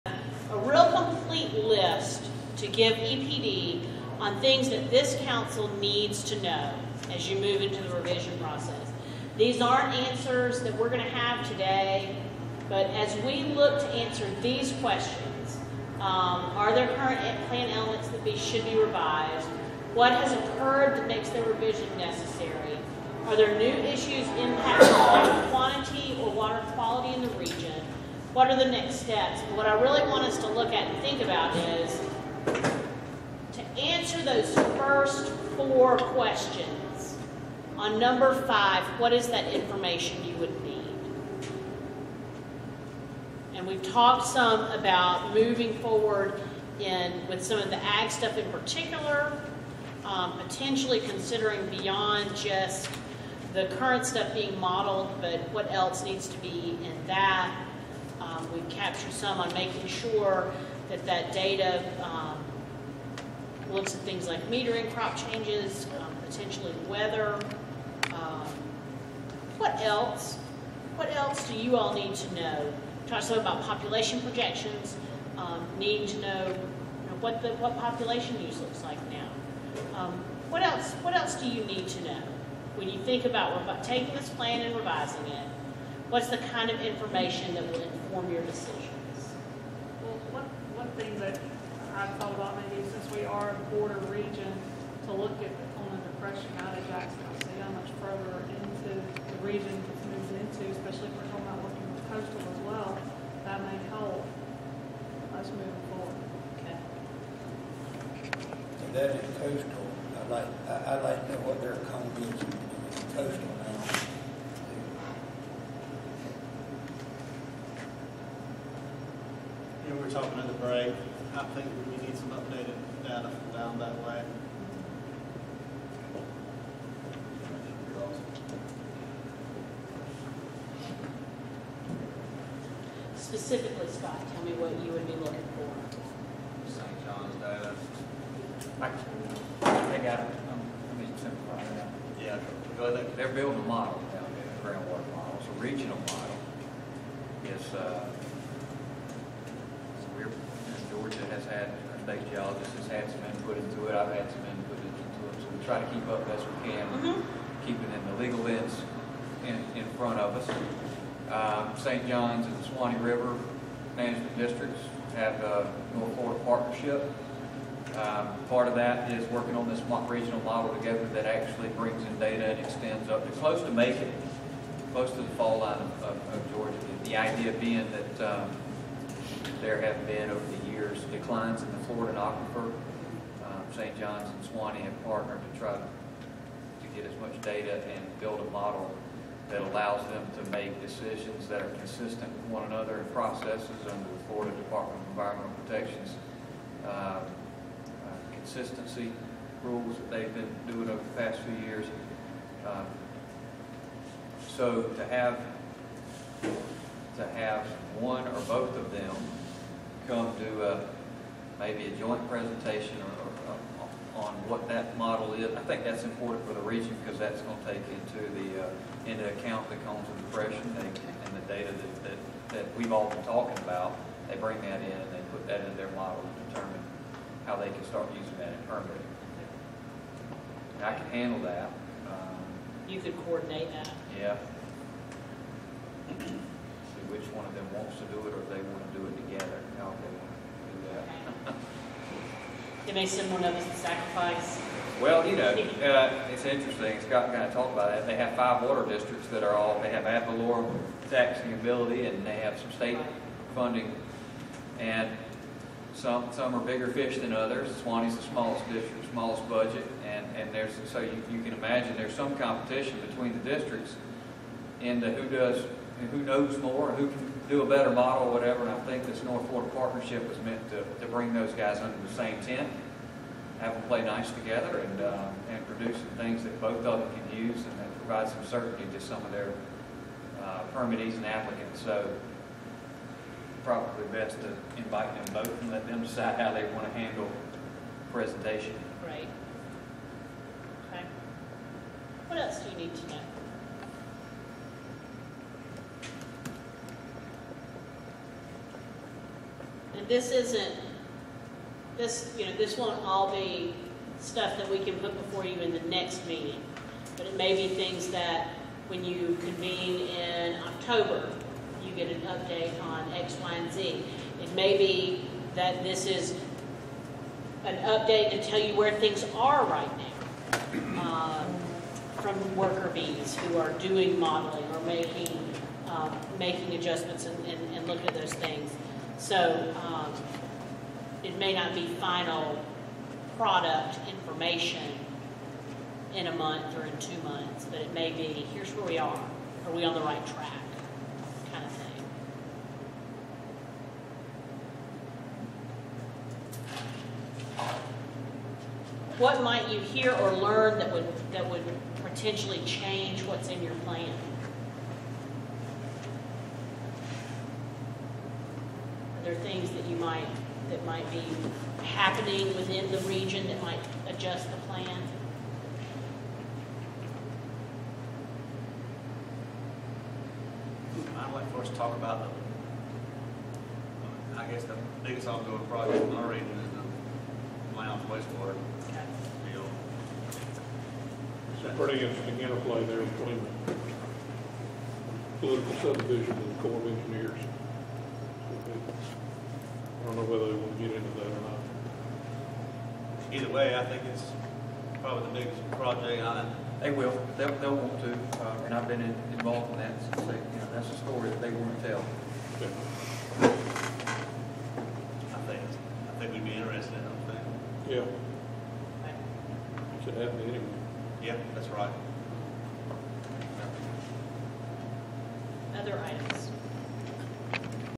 What this council needs to know @ SSRWPC 2015-06-15
Regular Session, Suwannee-Satilla Regional Water Planning Council (SSRWPC)